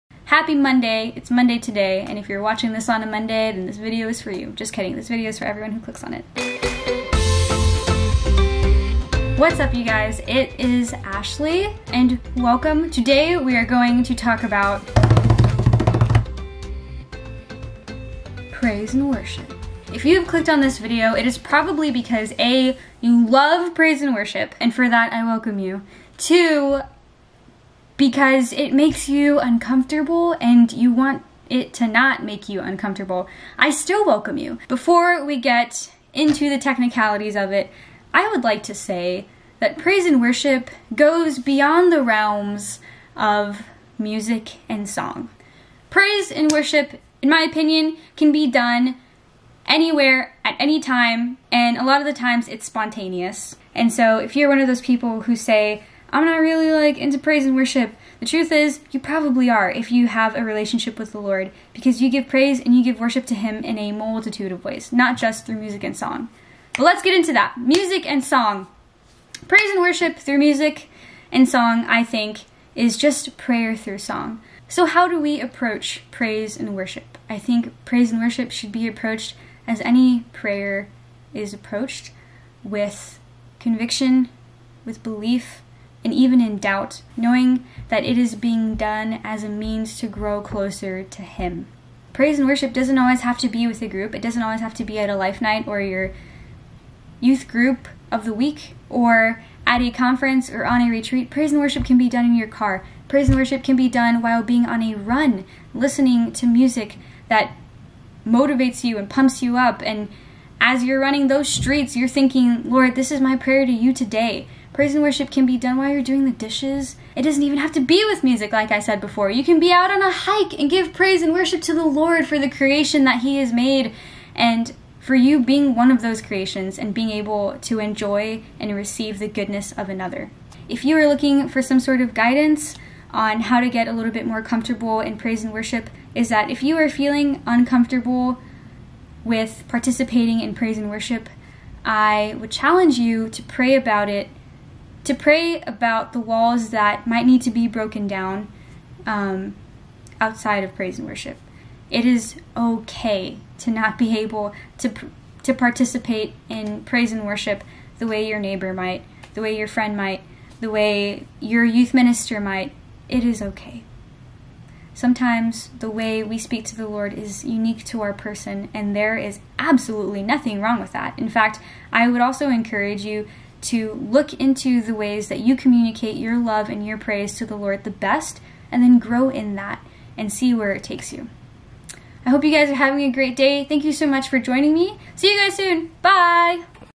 How to... praise & worship
Ashley Hinojosa is talking about what exactly praise and worship is all about... how to do it, how to be open to it, and how to glorify God in it.